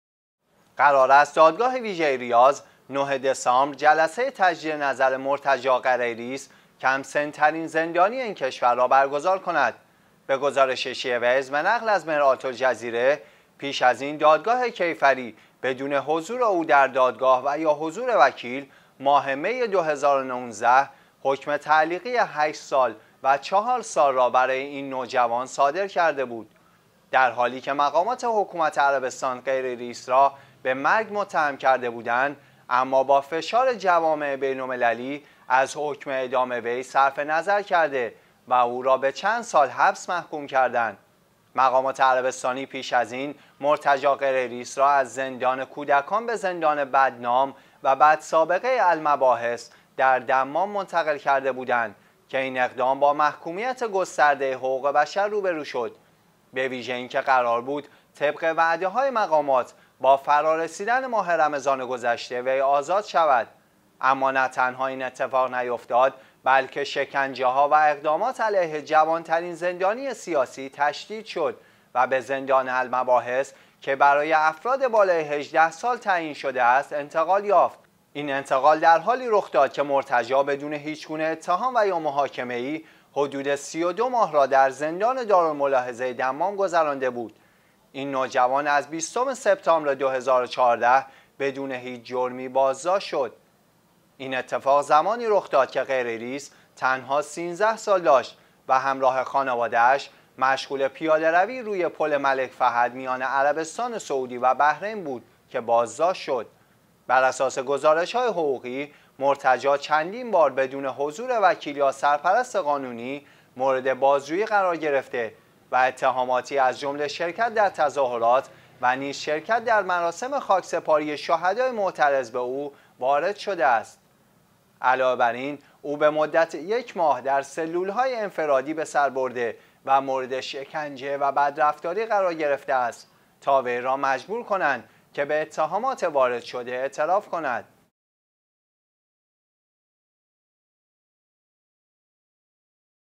عربستان محاکمه کم سن وسال ترین زندانی سیاسی را از سر می گیرد
قرار است دادگاه ویژه ریاض،  ۹ دسامبر  جلسه تجدید نظر مرتجی قریرص، کم‌سن ترین زندانی این کشور را  برگزار کند.
به گزارش شیعه ویوز به نقل از «مرآه الجزیره»، پیش از این دادگاه کیفری بدون حضور او در دادگاه و یا حضور وکیل، ماه مه ۲۰۱۹ ،  حکم تعلیقی ۸ سال و ۴ سال را برای این نوجوان صادر کرده بود.
در حالی که مقامات حکومت عربستان «قریرص» را به مرگ متهم کرده بودند؛ اما با فشار جوامع بین المللی، از حکم اعدام وی صرف نظر کرده و او را به چند سال حبس محکوم کردند.
مقامات عربستانی پیش از این، مرتجی قریرص را از زندان کودکان به زندان بدنام و بدسابقه «المباحث» در دمام منتقل کرده بودند که این اقدام با محکومیت گسترده حقوق بشر روبرو شد؛ به ویژه اینکه قرار بود طبق وعده های مقامات با فرا رسیدن ماه رمضان گذشته وی آزاد شود.
اما نه تنها این اتفاق نیفتاد بلکه شکنجه ها و اقدامات علیه جوانترین زندانی سیاسی تشدید شد و به زندان «المباحث» که برای افراد بالای هجده سال تعیین شده است انتقال یافت.
این انتقال در حالی رخ داد که «مرتجی» بدون هیچ گونه اتهام و یا محاکمه ای، حدود سی و دو ماه را در زندان «دار الملاحظه» دمام گذرانده بود.
این نوجوان از بیستم سبتامبر ۲۰۱۴ بدون هیچ جرمی بازداشت شد. این اتفاق زمانی رخ داد که «قریرص» تنها سیزده سال داشت و همراه خانواده اش مشغول پیاده روی روی پل ملک فهد میان عربستان سعودی و بحرین بود که بازداشت شد.
براساس گزارش های حقوقی، مرتجی چندین بار بدون حضور وکیل یا سرپرست قانونی مورد بازجویی قرار گرفته، و اتهاماتی از جمله شرکت در تظاهرات و نیز شرکت در مراسم خاکسپاری شهدای معترض به او وارد شده است.
علاوه بر این او به مدت یک ماه در سلول های انفرادی به سر  برده و مورد شکنجه و بدرفتاری قرار گرفته است تا وی را مجبور کنند که به اتهامات وارد شده اعتراف کند.